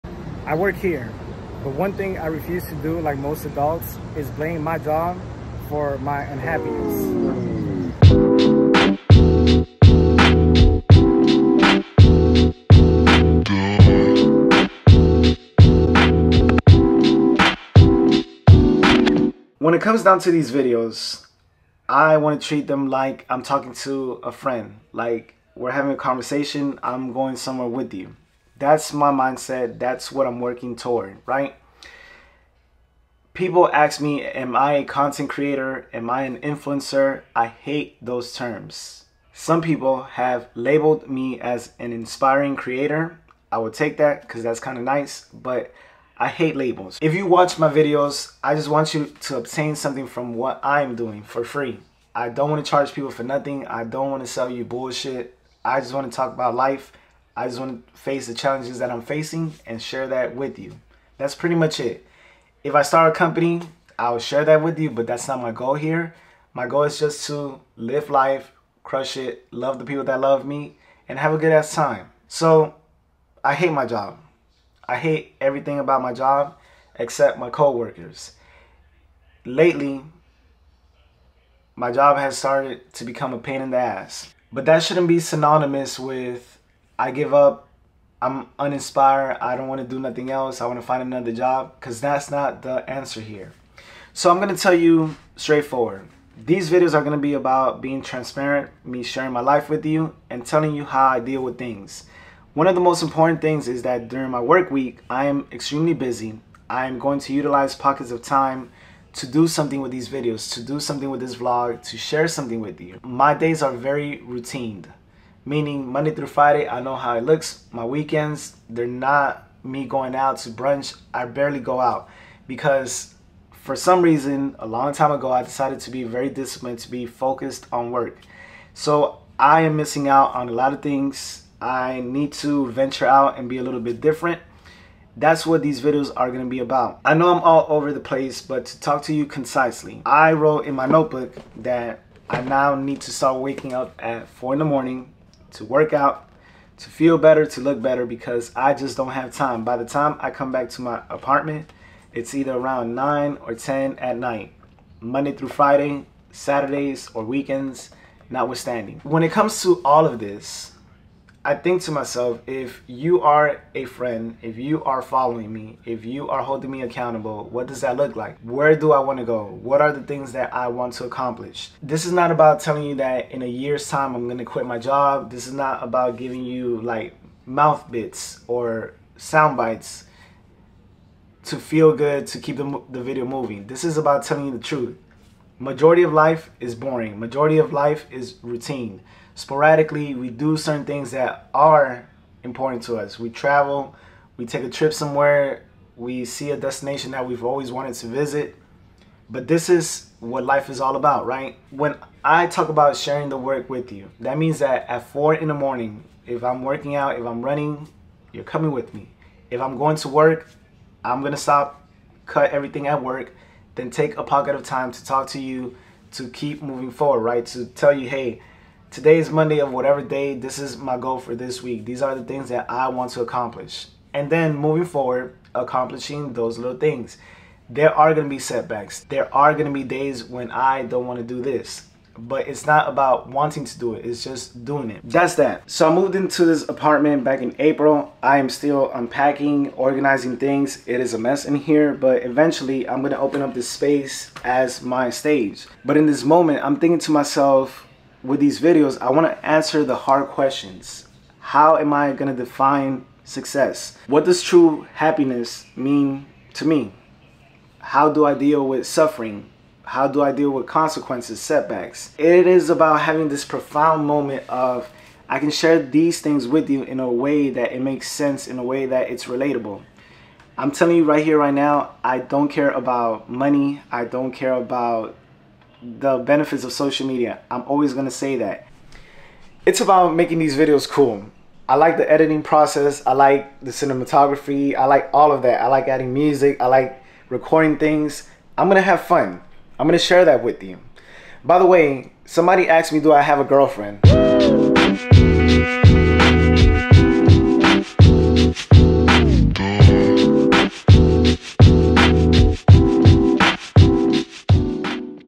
TRUTH is: I HATE MY JOB | Episode 3 – The Daily Vlog
Balancing professional dissatisfaction with unwavering dedication is a profound testament to personal integrity. Despite hating my job, I remain steadfast in my responsibilities, driven by both professional and personal obligations. This dual commitment demands that I hold myself to the highest standards of accountability, ensuring excellence in every task. It is a challenging yet deeply honorable journey, where the pursuit of responsibility and integrity transcends personal discontent, showcasing a resilient spirit and an enduring commitment to doing what is right.